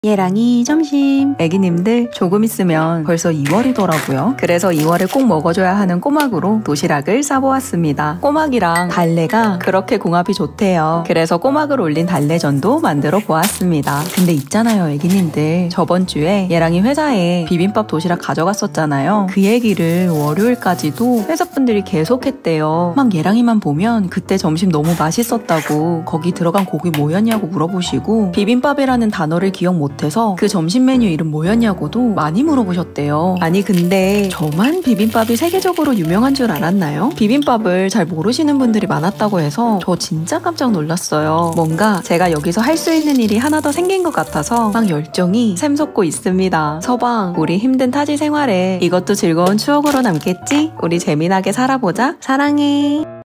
비빔밥 이미 한 20년 전부터 유명한 거 아니였나요...? 띠로리...ㅎㅎㅎ
애기님들 2월에 꼬막 제철이라
생물사다가 데쳐 드실꺼죠?ㅠㅠ
넘나 부럽습니다♡

미국에서.. 구할 수 있는데
제가 못 구하는 건지...
꼬막이! 우선 냉동뿐이 없더라구요...
꼬막 살짝만 데쳐서
쏙 빼 먹으면 너무너무 맛있잖아요ㅠㅠ♡

꼬막이 마늘이랑 달래랑
음식궁합 잘 맞는다구 하니까
마늘, 달래 듬뿍 넣어서
꼬막달래장 만들어 드시는 것도
넘나 추천드려용♡

그리구
비빔밥의 인기는
오랫동안 식지 않았다구 합니다ㅎㅎㅎ
2월에는 설날도시락 싸면서
우리 음력설에 이런거 먹어용~ 하려구용!!!
벌써 메뉴 구성중이랍니다 히히히♡

영상으로도 애기님들께 꼭 보여드릴께용?♡
그럼 편안한 하루 보내떼요♡

라뷰♡

♥꼬막달래전♥
1. 달래가 풀리지 않도록 돌돌 말아주고♡
2. 달래 팬에 올리구 부침물(부침가루50g+물100ml) 쏘~옥 넣어주기♡
3. 꼬막 올려서 노릇노릇하게 구워주기♡
*달래는 너무 많이 꼬지 말아주세용 많이 꼬시면 보기는 좋은데 질겨서 먹기 힘드니까 살살만 꼬아 주시어요♡

♥콩나물무침♥
1. 소금(1T) 넣은 끓는 물에 콩나물(340g) 3분 데쳐서 조금 식혔다가♡
2. 양념장 버무리기♡
-콩나물(340g)
-멸치 액젓(1T)
-알룰로스(1/2T)
-고춧가루(1T)
-다진 마늘(1T)
-참기름(1T)
-깨 톡톡

♥꼬막비빔밥 양념장♥
-꼬막(340g)
-간장(2T)
-미청(2T)
-알룰로스(1T)
-고춧가루(1T)
-다진마늘(1.5T)
-참기름(2T)
-양파(소) 1/2개
-청양고추 1개(매콤하게 드시고 싶으시다면♡)
-파 1대
-통깨